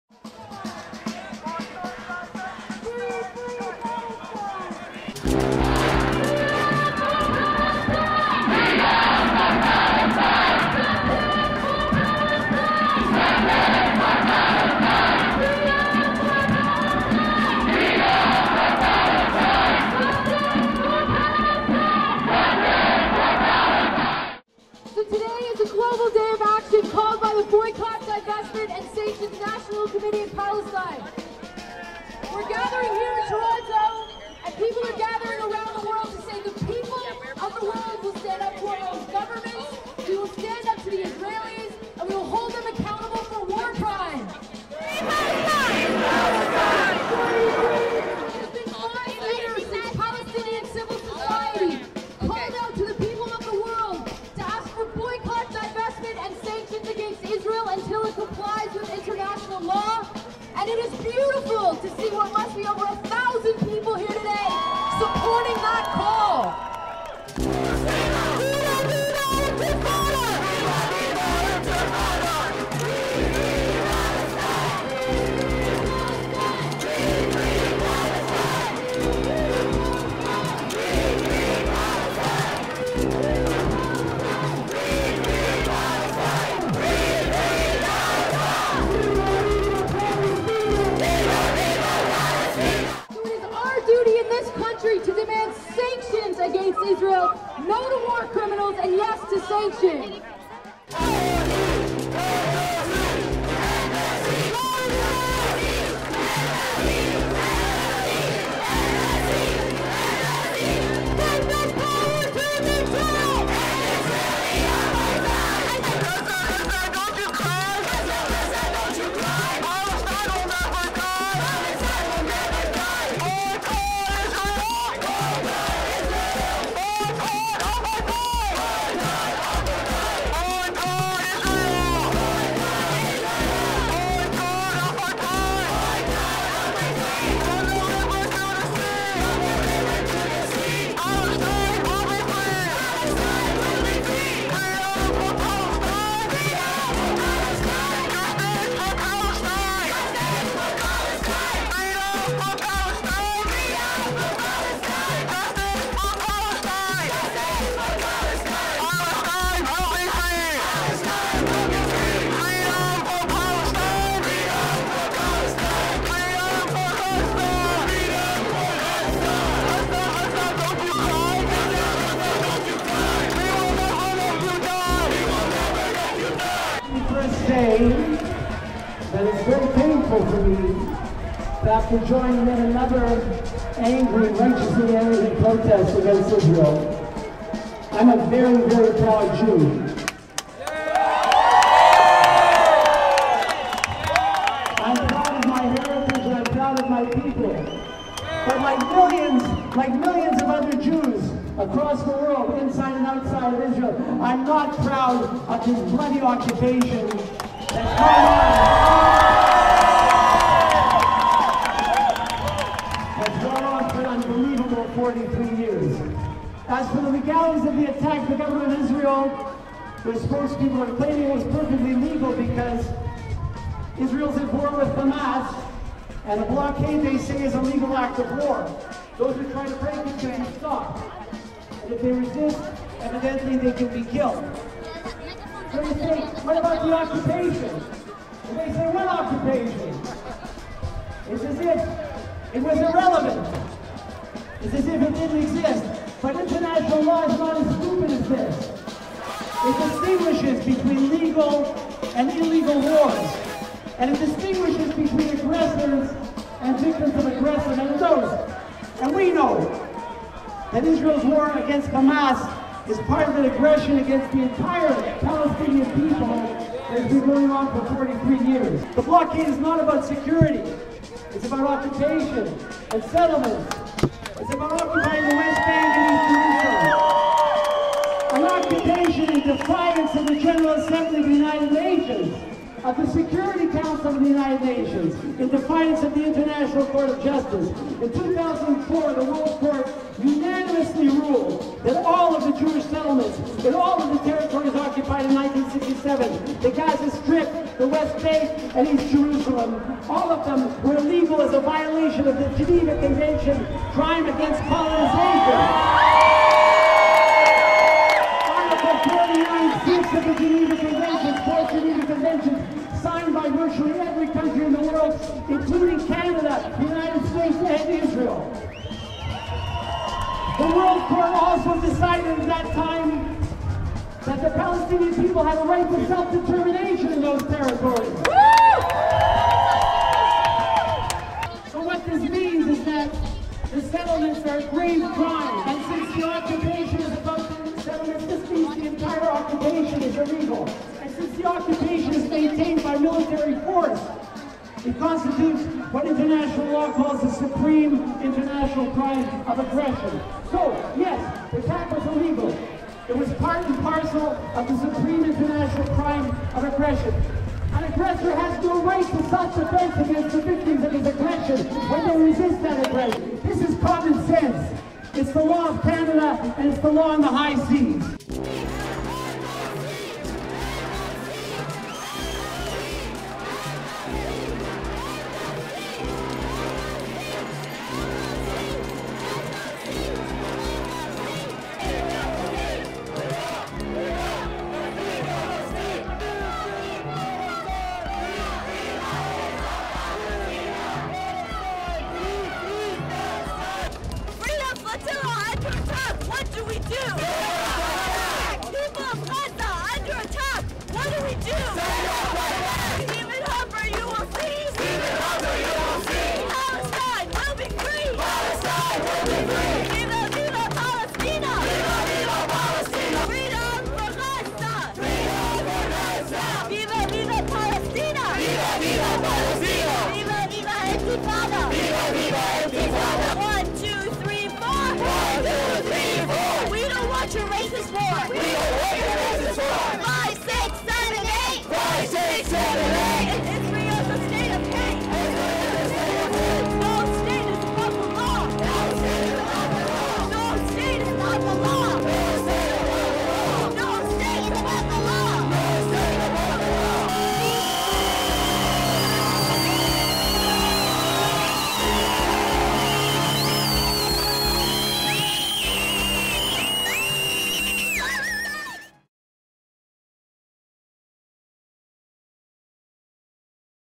Gaza Freedom Flotilla March
On Saturday, June 5, 2010, human rights and community organizations mobilized to join an emergency Global Boycott Divestment Sanctions (BDS) Day of Action called by the Palestinian Boycott, Divestment and Sanctions National Committee (BNC).

In Toronto, protesters condemned the fatal attacks by Israel on the Gaza Freedom Flotilla carrying humanitarian aid to the occupied and besieged Gaza Strip.

Organized by:

Coalition Against Israeli Apartheid
Palestine House Community Centre
Canadian Arab Federation
Toronto Coalition to Stop the War
Canadian Peace Alliance